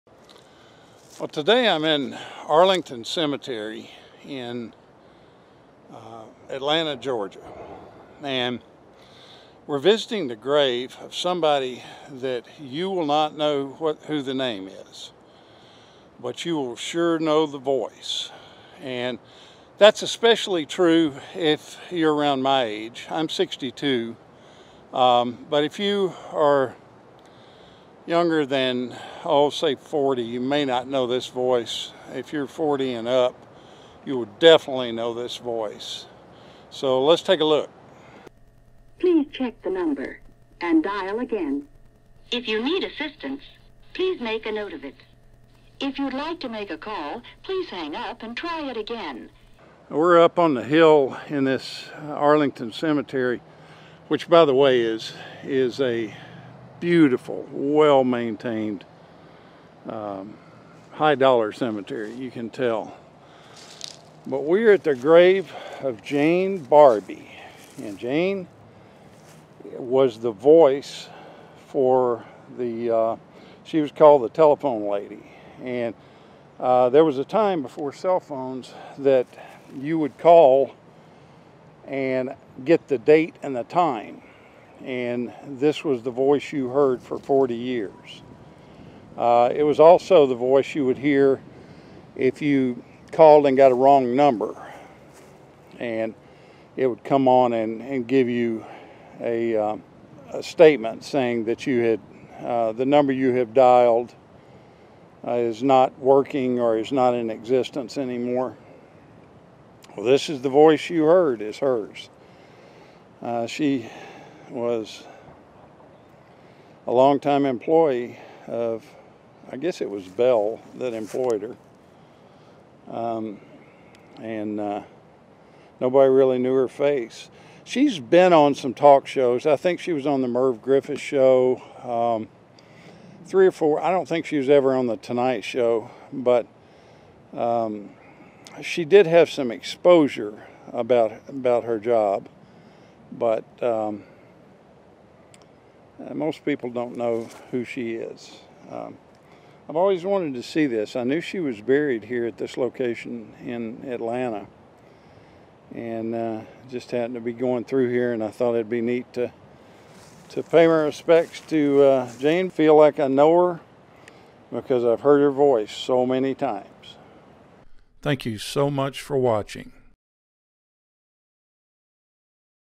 The GREATEST TELEPHONE VOICE ever!
A visit to the final resting place of Jane Barbie AKA "The telephone lady",  who's voice was used for over 40 years as the "time and temp" number you could call.